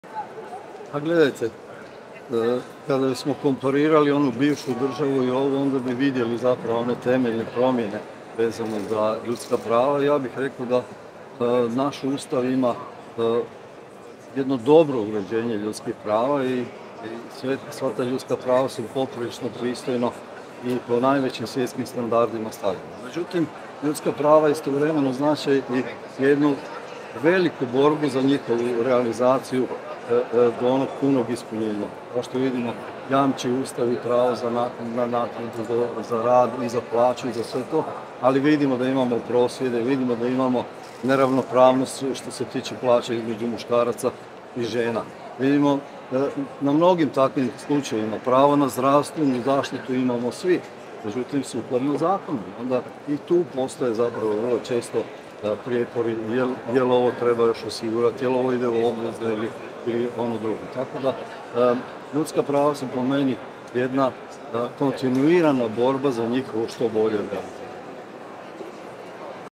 Dražen Bošnjaković o ljudskim pravima u Hrvatskoj
Konferencija povodom 75 godina od donošenja Opce deklaracije o ljudskim pravima Ujedinjenih naroda održana je u Velikom predvorju Nacionalne i sveučilišne knjižnice u Zagrebu.